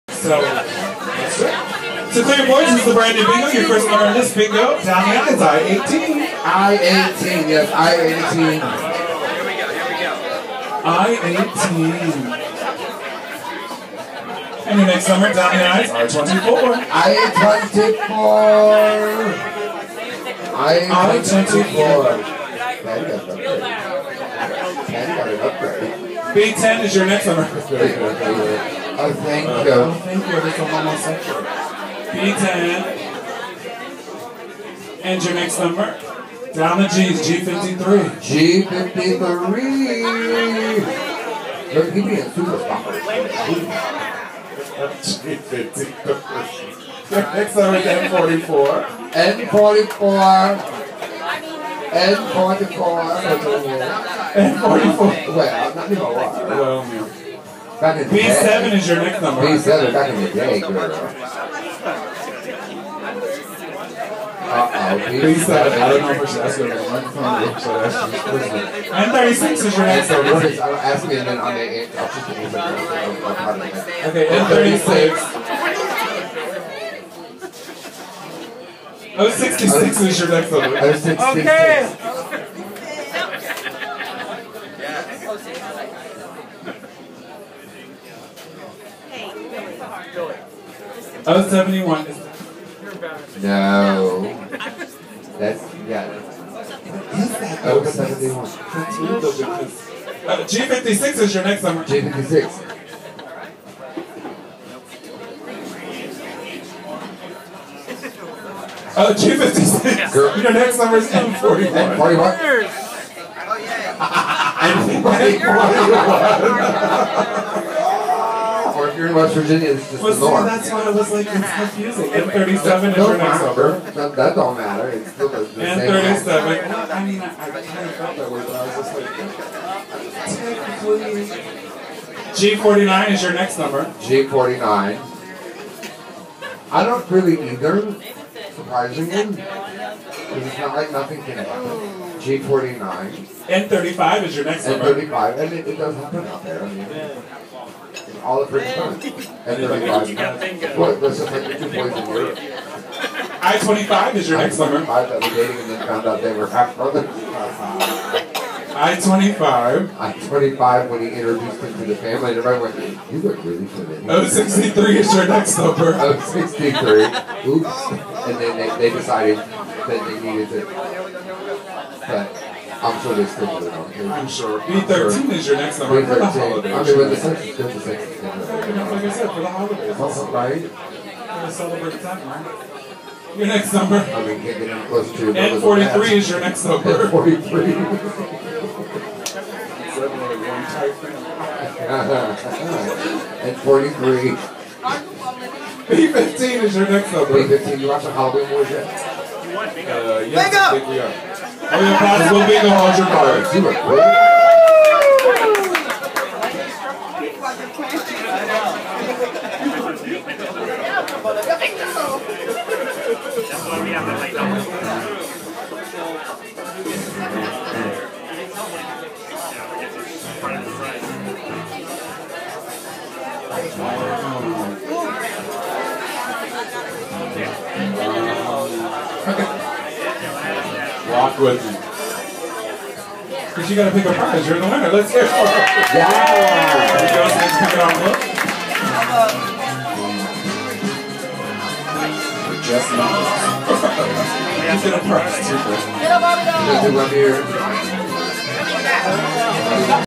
October 2018 Bingo @ Freddie's Beach Bar & Restaurant
Here's a video clip from bingo last night. Bingo happens every week on Wednesdays at 8 pm, so be sure to place this on your schedule. Please be sure to reserve a table in advance.